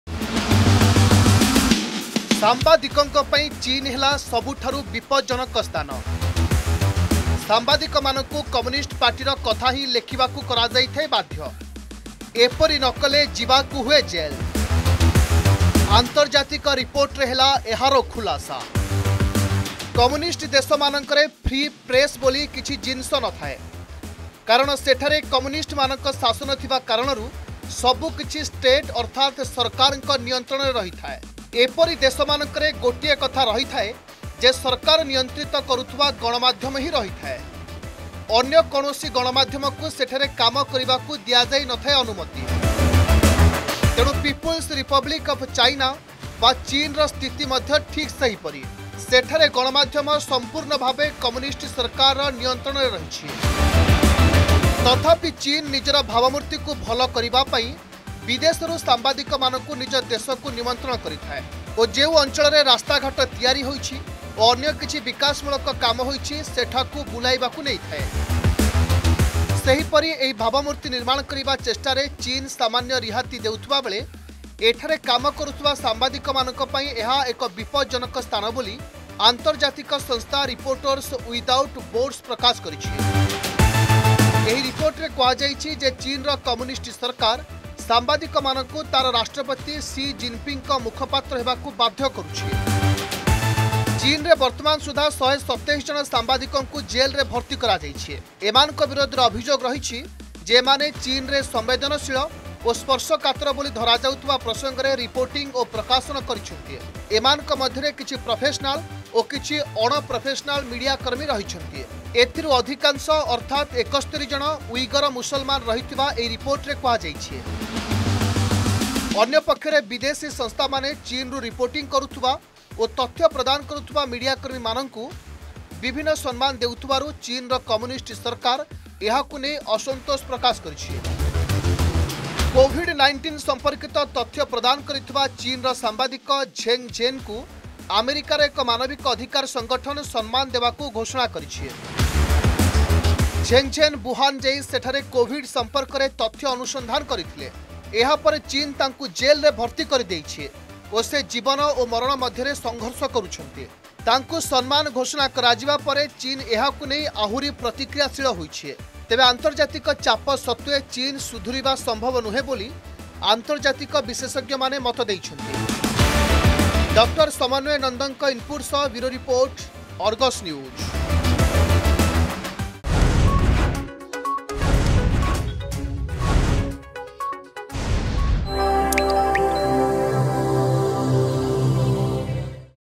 ସାମ୍ବାଦିକଙ୍କ ପାଇଁ ଚୀନ ହେଲା ସବୁଠାରୁ ବିପଜ୍ଜନକ ସ୍ଥାନ
ସାମ୍ବାଦିକ ମାନଙ୍କୁ କମ୍ୟୁନିଷ୍ଟ ପାର୍ଟିର କଥା ହିଁ ଲେଖିବାକୁ କରାଯାଇଥାଏ ବାଧ୍ୟ ,ଏପରି ନ କଲେ ପୂରା ଯାଇଥାଏ ଜେଲରେ । ଆନ୍ତର୍ଜାତିକ ରିପୋର୍ଟରେ ହେଲା ଏହାର ଖୁଲାସା ।